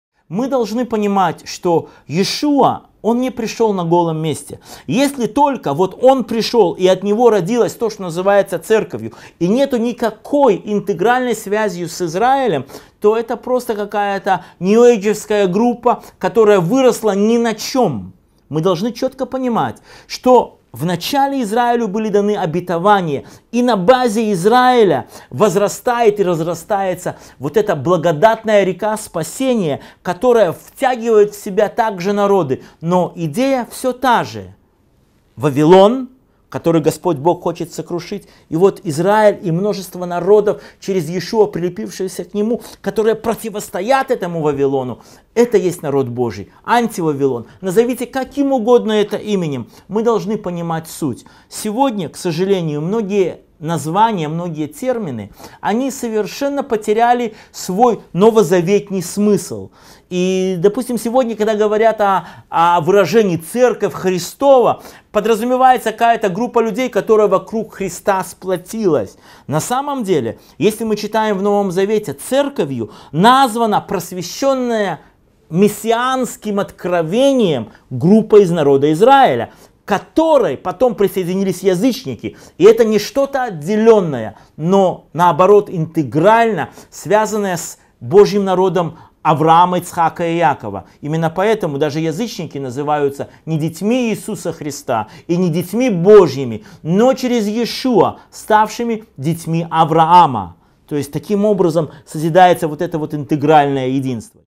Связь церкви с Израилем (Леон Мазин)
Если у церкви нет связи с Израилем, то это просто группа, которая выросла ни на чем.
Вначале Израилю были даны обетования, и на базе Израиля разрастается эта благодатная река спасения, которая втягивает в себя также и другие народы (через Иешуа).

Церковь - это просвещенная мессианским откровением группа из народа Израиля, к которой потом присоединились язычники.

Вы можете посмотреть все части цикла "Еврейский взгляд на книгу Откровение"